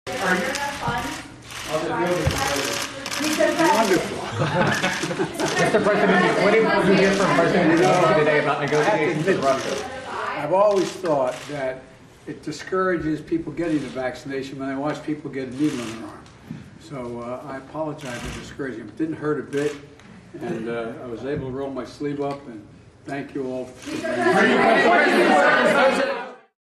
WEB EXTRA: President Biden Receives Second COVID-19 Booster
President Biden received his second COVID-19 booster shot after the FDA approved a second booster for people 50 and older. On Wednesday (3/30), the president said, "I've always thought that it discourages people getting the vaccination when they watch people get a needle in their arm, so I apologize for discouraging. But it didn't hurt a bit."